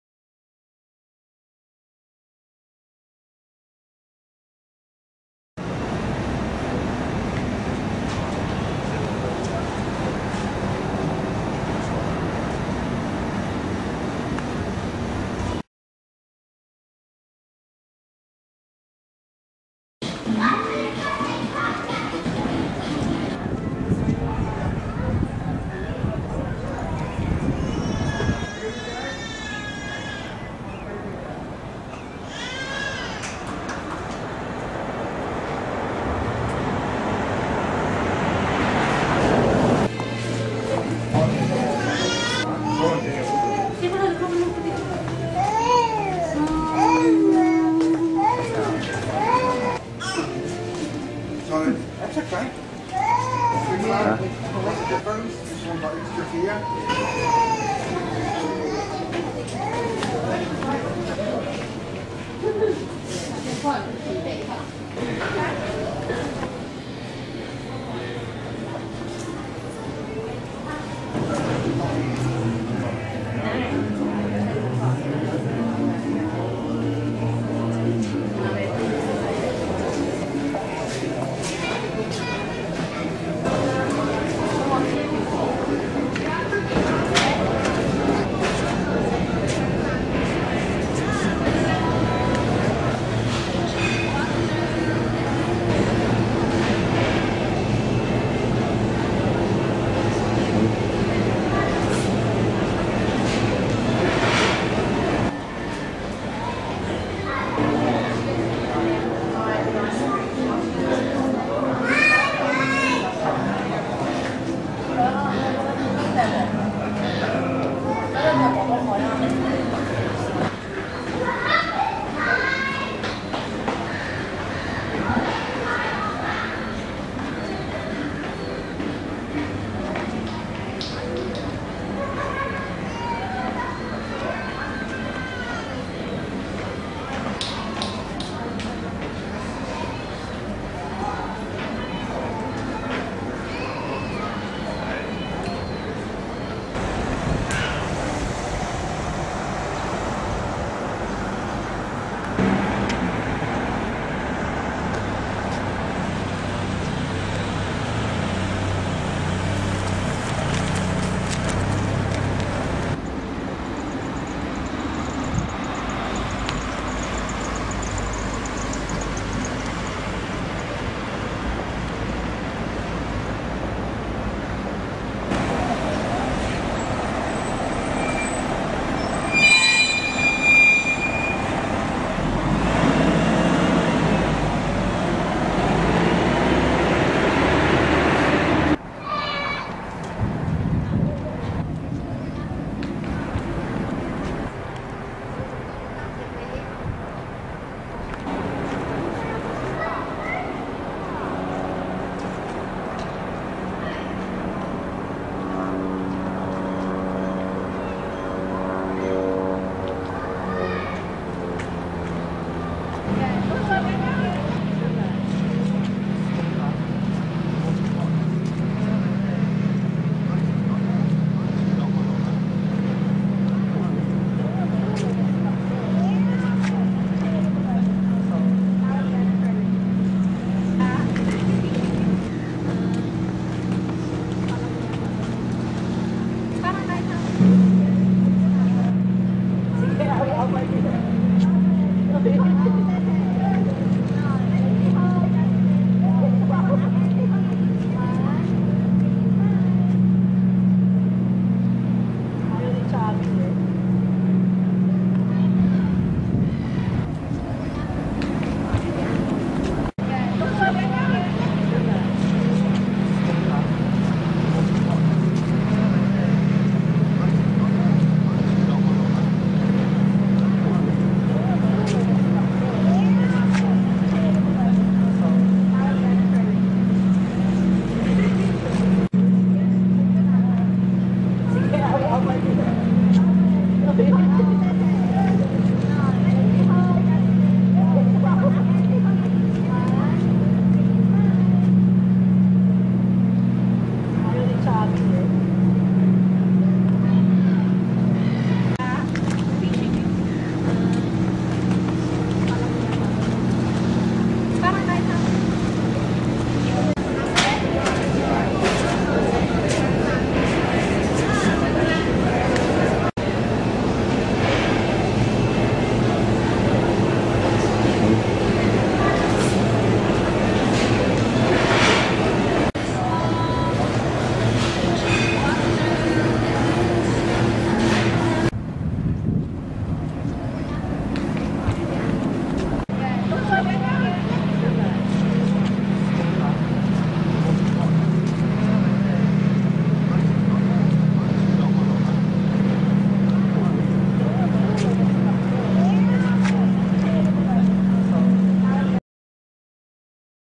E Shed Tourist Markets, Fremantle Western Australia
E Shed Tourist Markets, Fremantle   Western Australia.  You can find all kinds of souvenirs, including fresh food and quality foods here.  Recommended to visit.